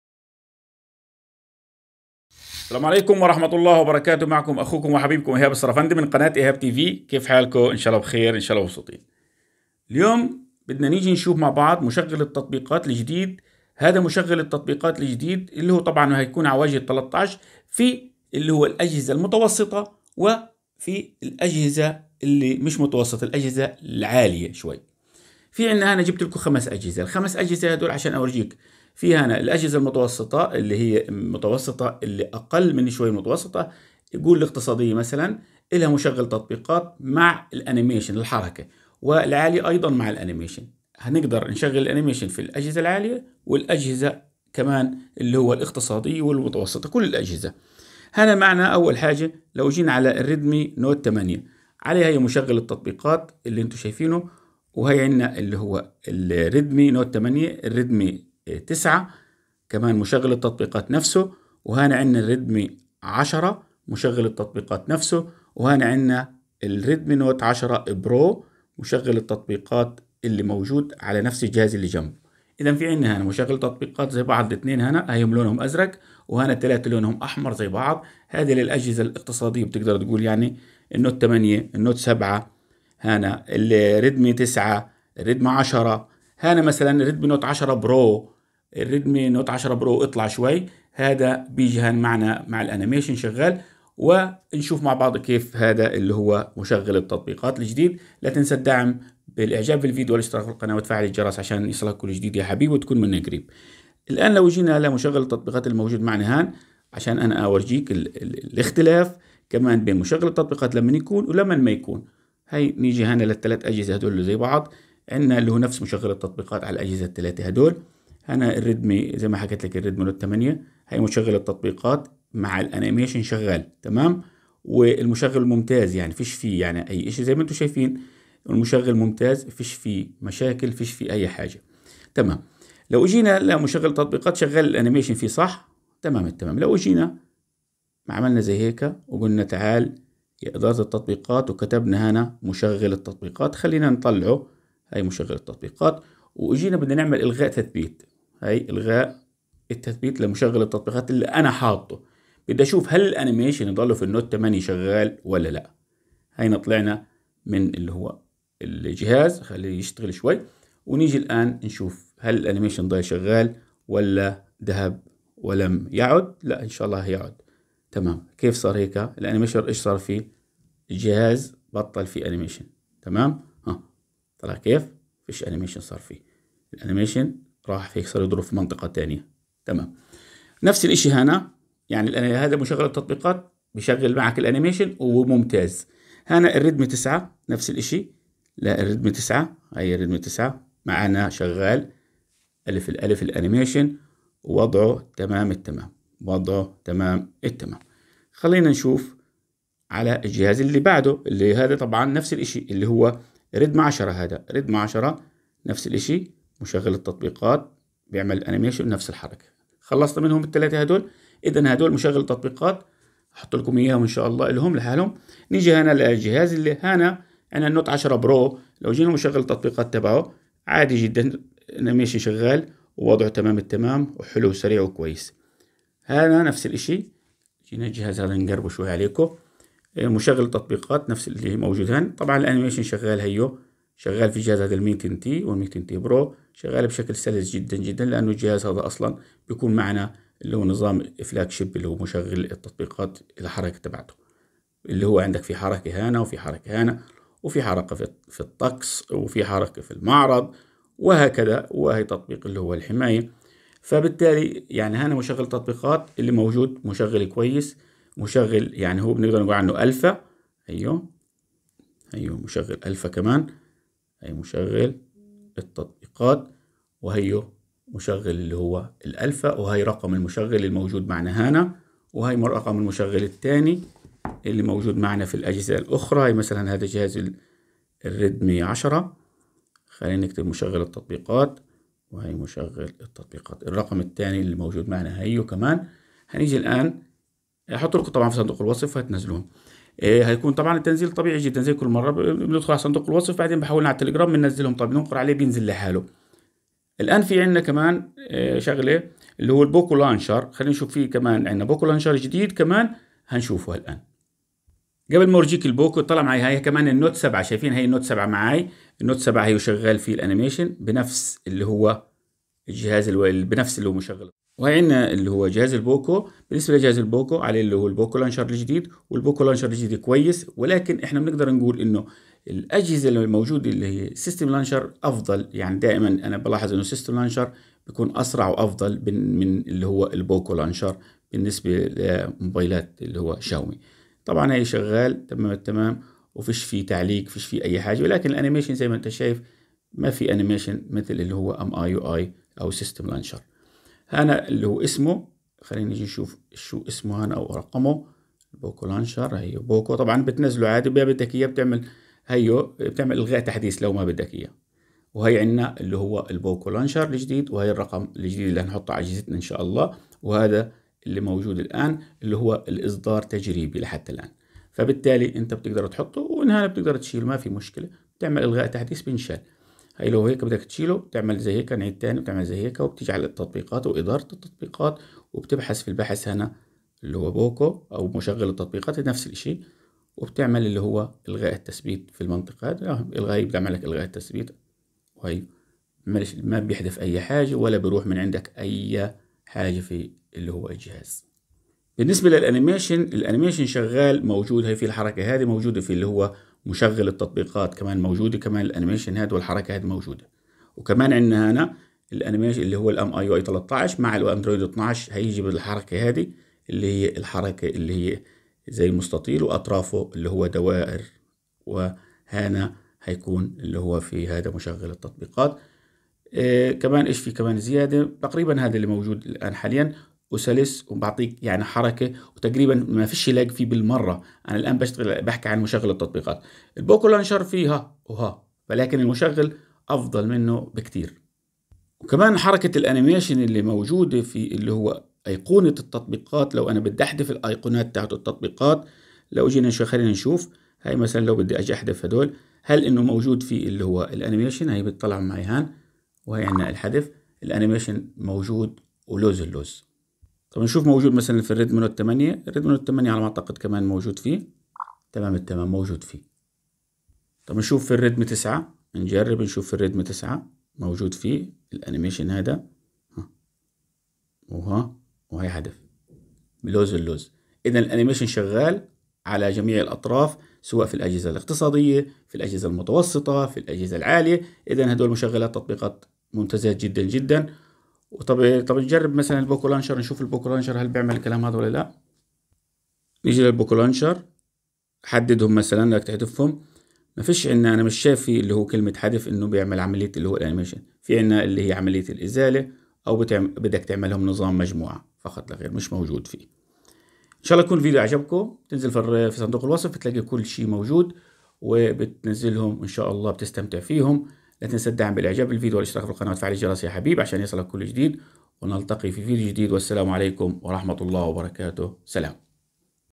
مشغل التطبيقات السلس والمحسن MIUI 13 الأنيميشن شغال في جميع الأجهزة🔥 وتحديث البوكو لانشر الجديد معهم
لجميع هواتف شاومي وريدمي وبوكو مع مشغل التطبيقات  MIUI 13 لجميع هواتف شاومي وريدمي الأجهزة الاقتصادية والمتوسطة والعالية وايضا المشغل الجديد للبوكو لانشر POCO LAUNCHER